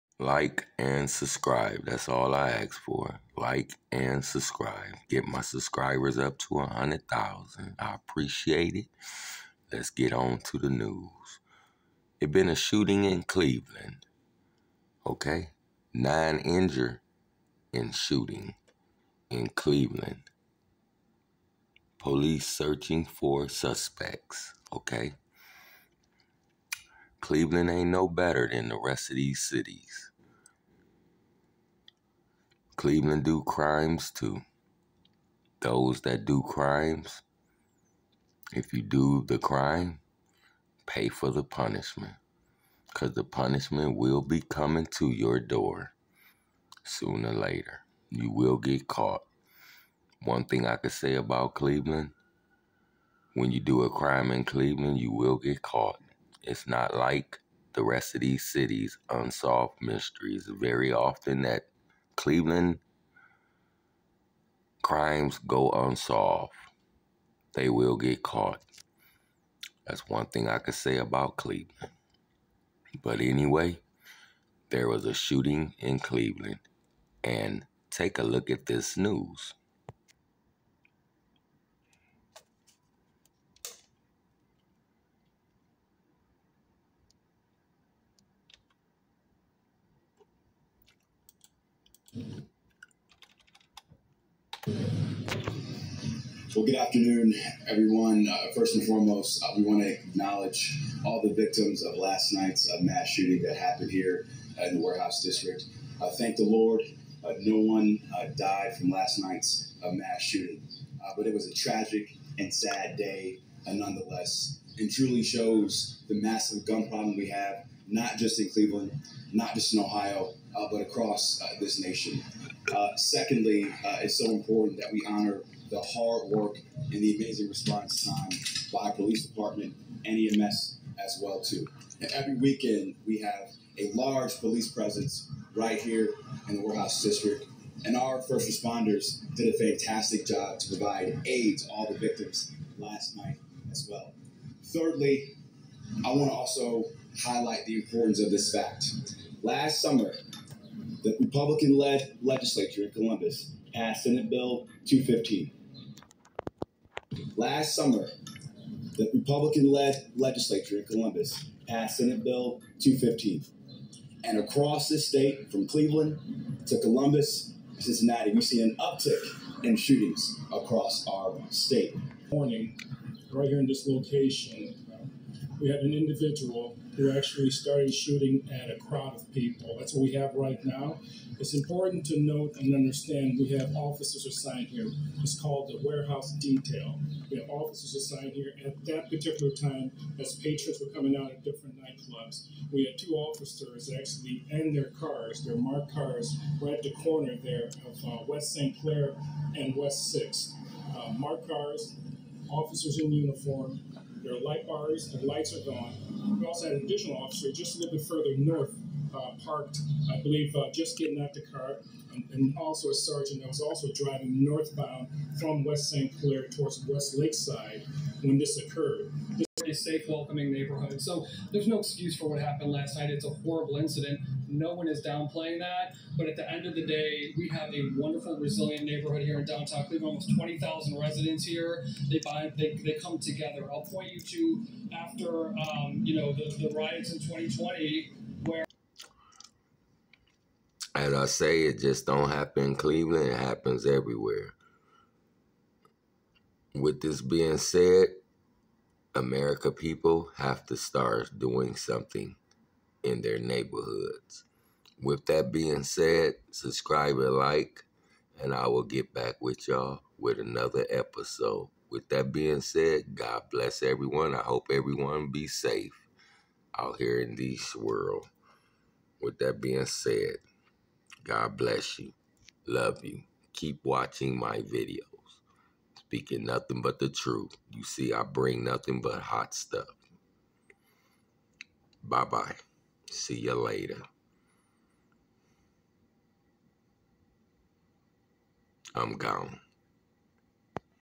Cleveland random shooting, check this video out....
WOW, this happened in Cleveland Ohio, something needs to stop it's getting ridiculous what's happening in all these cities.